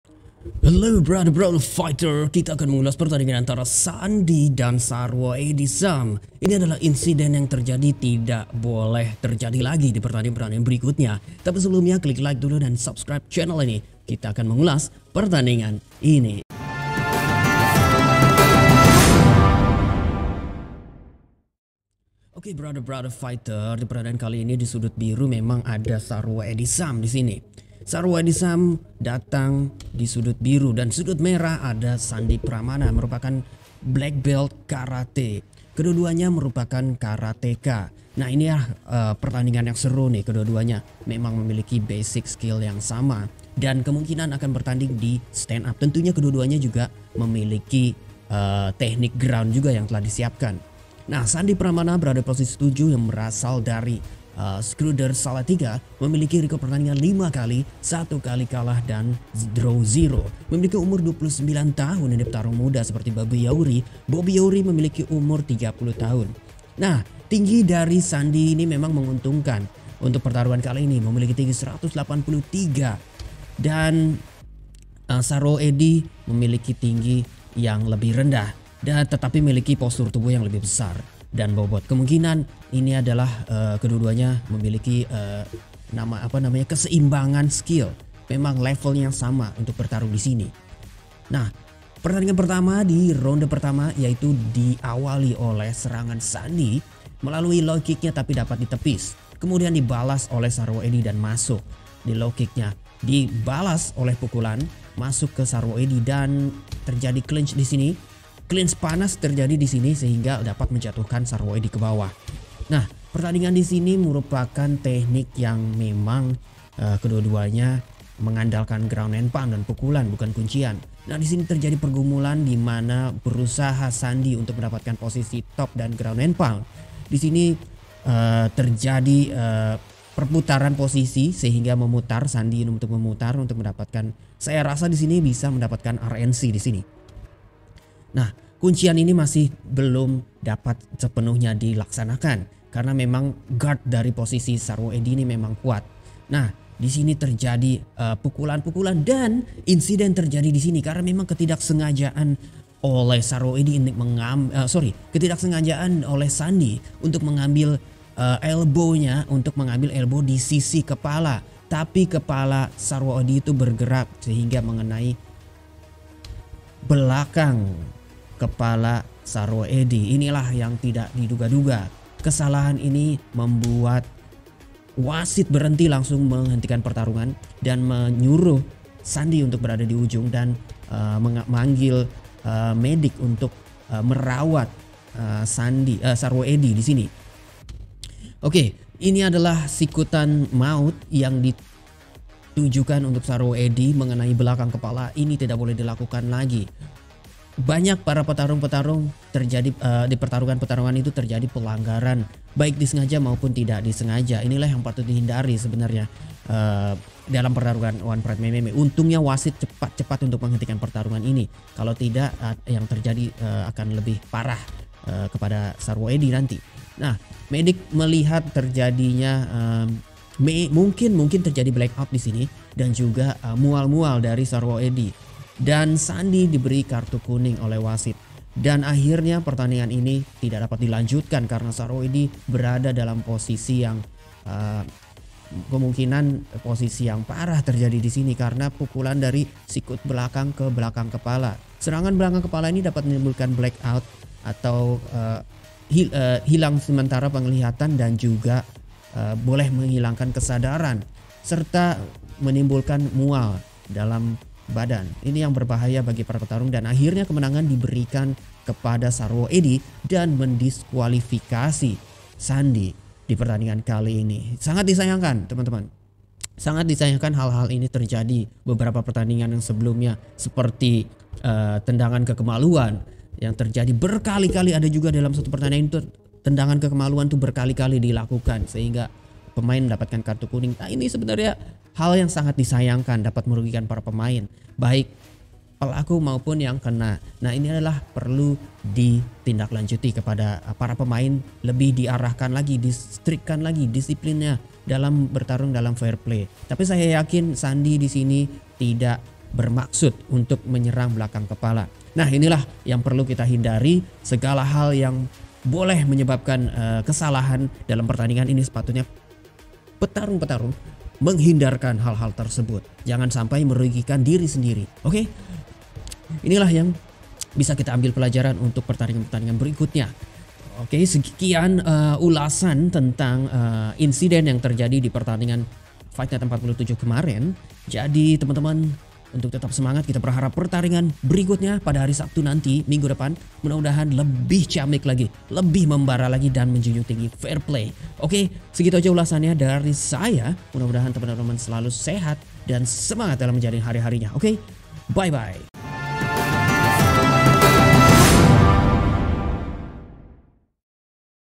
INSIDEN PERTANDINGAN SARWO EDI VS SANDI - FN47
Karena memukul ke arah kepala belakang, Sandi Permana harus di diskualifikasi. Join membership Youtube One Pride MMA untuk full videonya. OnePrideMMA OnePrideFightNight47 . M. Sandi Permana dinyatakan di diskualifikasi karena memukul kepala bagian belakang dari Sarwo Edhi. Sarwo Edhi pun berhak untuk meraih kemenangannya.